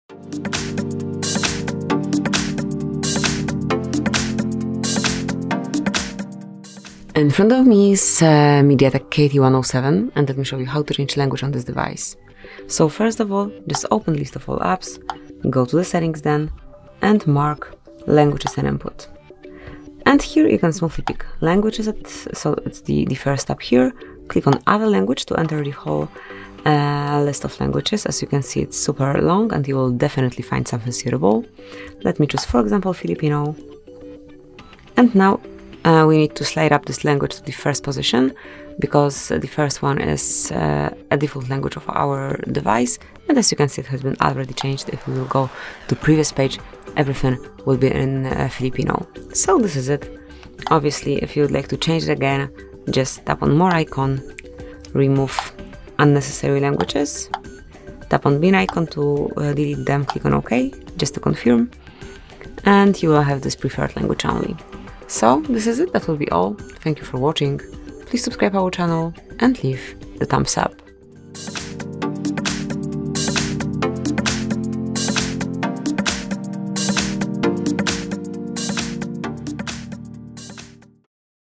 How to Change Language in MEDIATEK KT107 – Device Language
If you want to know how to change the language in MEDIATEK KT107, open this tutorial. Our expert will show you where the language settings are located in MEDIATEK KT107 and how to successfully set a new default language in MEDIATEK KT107.